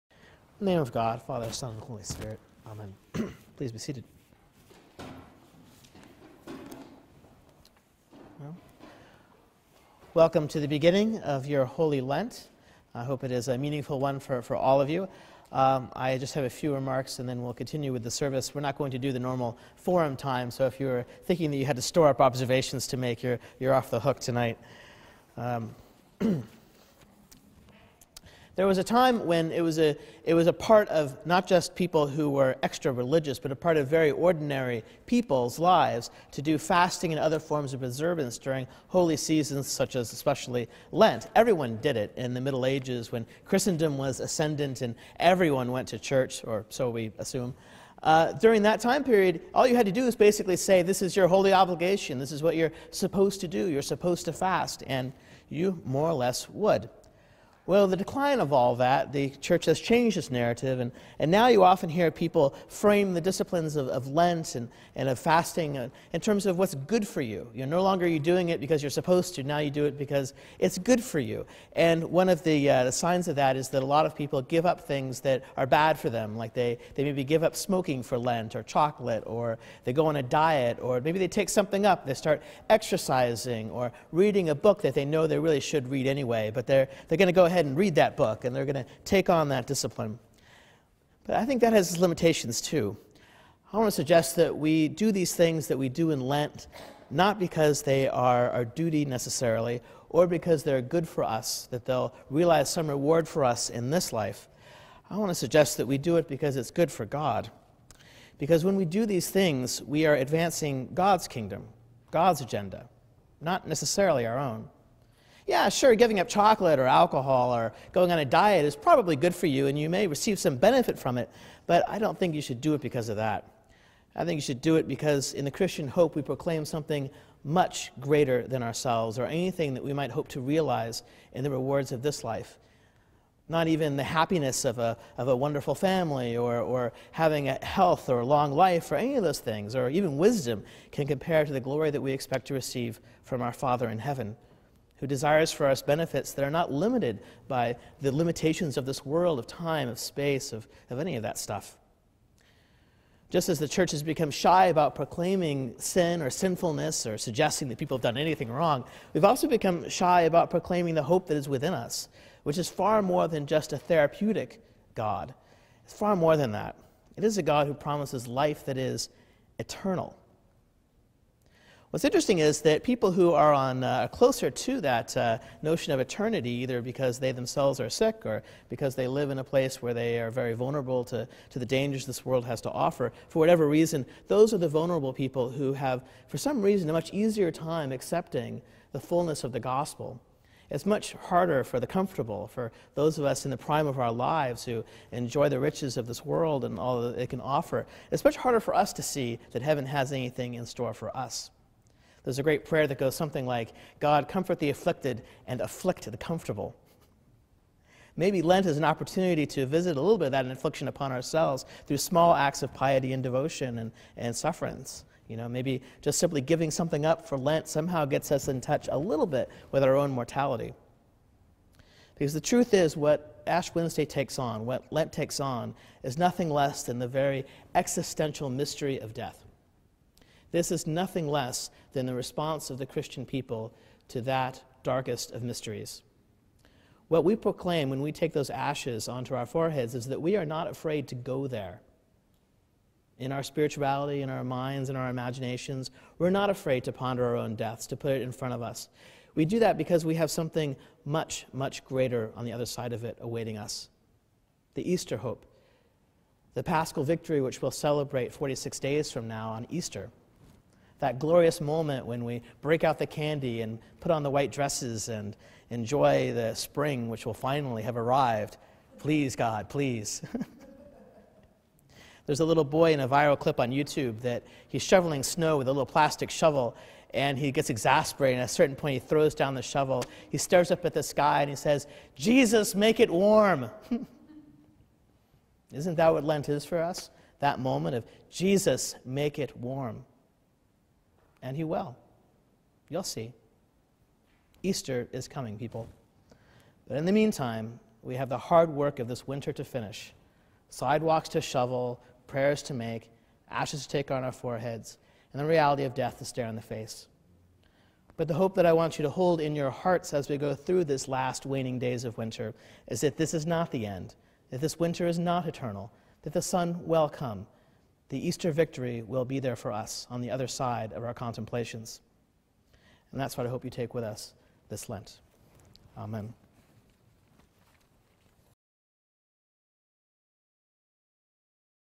Sermon: Ash Wednesday 2014
"Jesus, make it warm!" cries the little boy in the viral video hit of this winter. Spring is coming, but before it does we still have to slog through the last days of spiritual winter dealing with the hard fact of death. We should undertake the spiritual disciplines of Lent not out of obligation or because they are "good for us," but because they are good for God--they advance His agenda for our broken and cold world. Christians have become shy not only in decrying sin but in proclaiming the hope that is in us, and Lent gives us an opportunity to address both of those deficits.